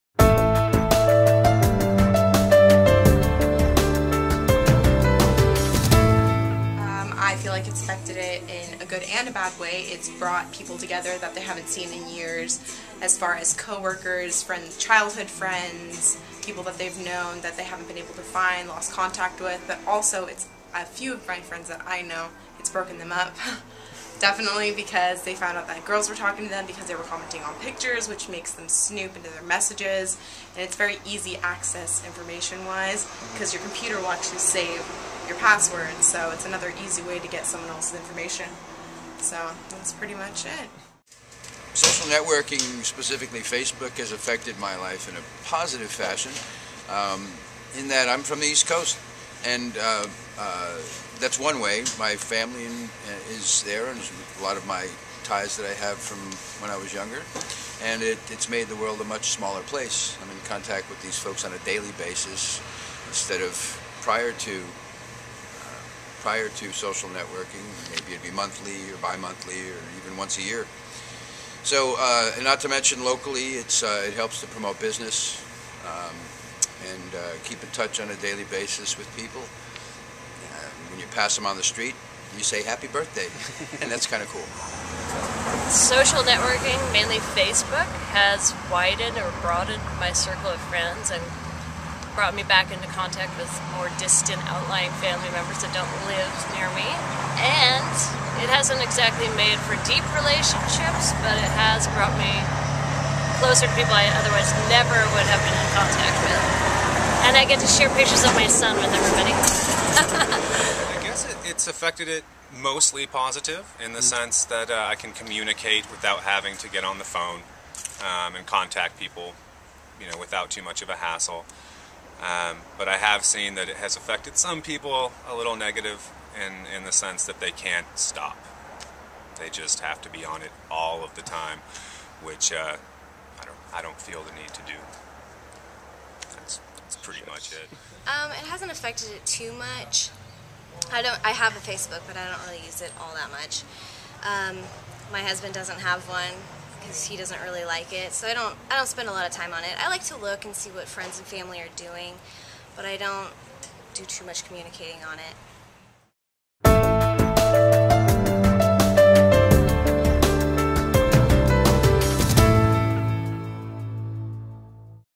OVN Sidewalk Talk Social Media affect you?
How has social networking affected your relationships? 1/27/2012